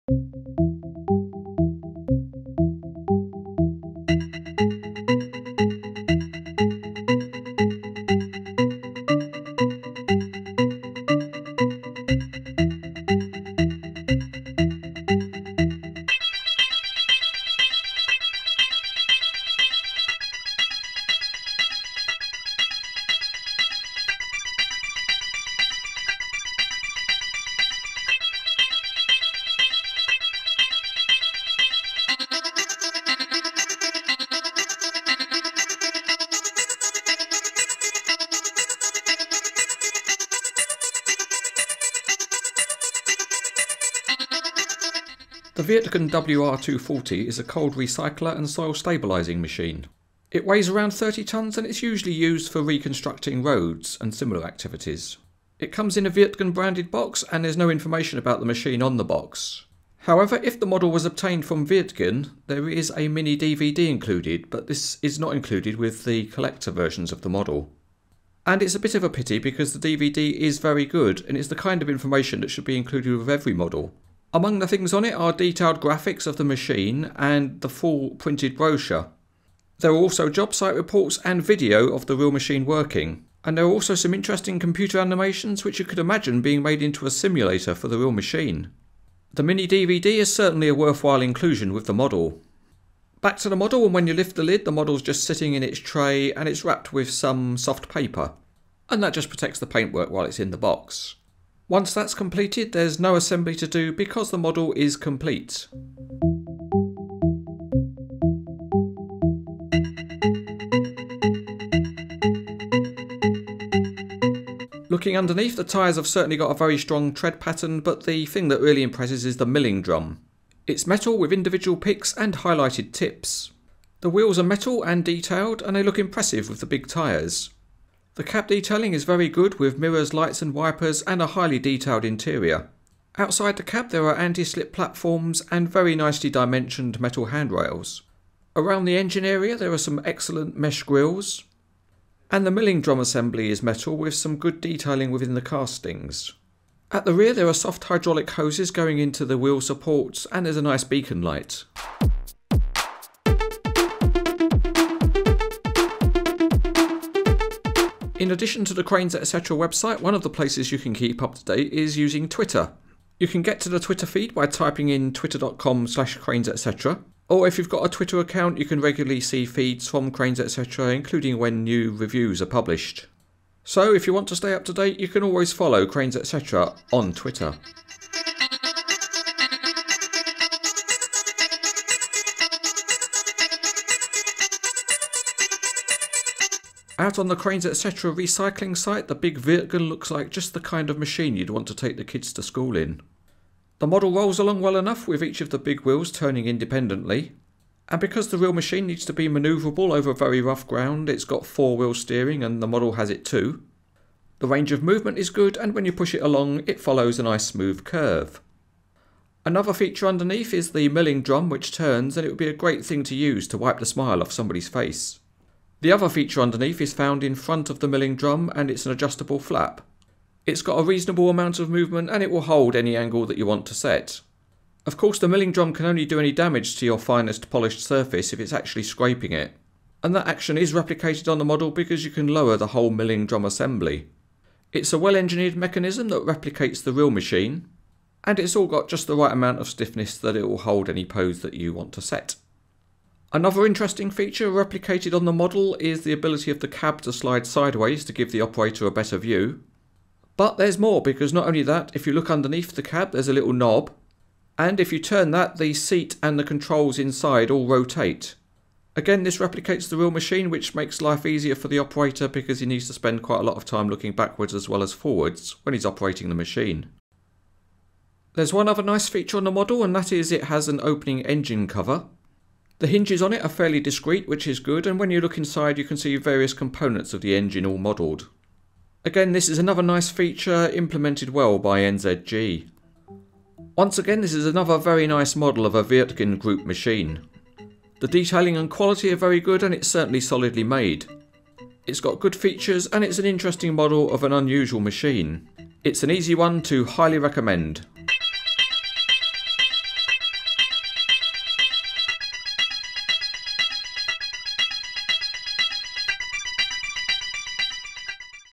NZG Wirtgen WR 240 Cold Recycler and Soil Stabiliser by Cranes Etc TV
This is the Cranes Etc TV review of NZG's 1/50 scale model of the Wirtgen WR 240 Cold Recycler and Soil Stabiliser .  The model number is 871.  The full review is on the Cranes Etc